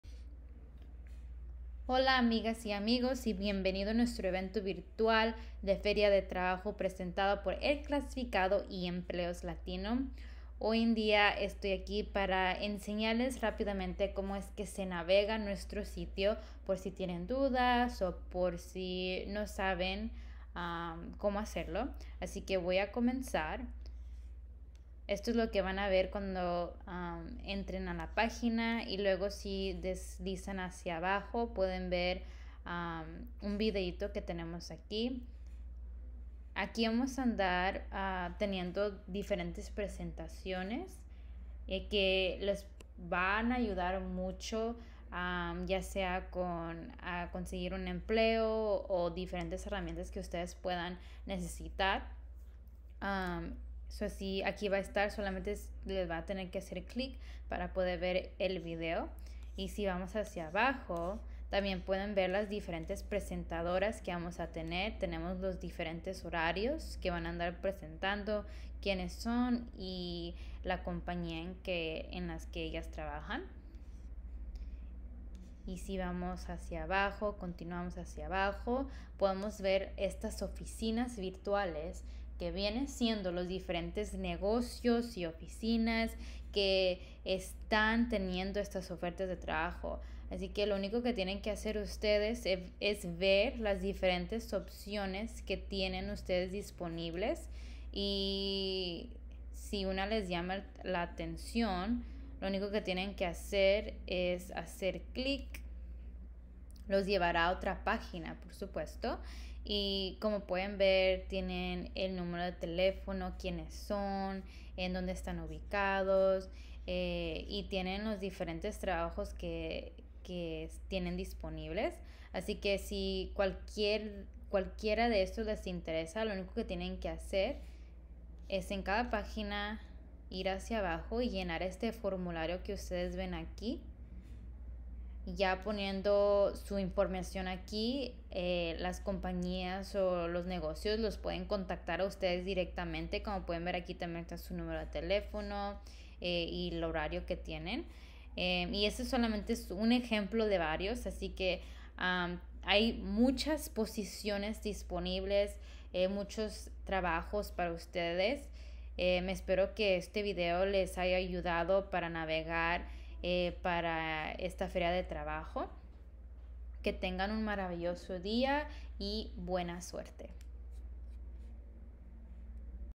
Feria de trabajo 2021 - Como Navegar Nuestra Pagina
Bienvenidos a nuestro evento Virtual de Feria de trabajo.

Tenemos muchas opciones de ofertas de trabajos disponibles para todos ustedes. Para saber cómo navegar y conseguir el empleo deseado, llene el formulario que enseñamos durante esta presentación.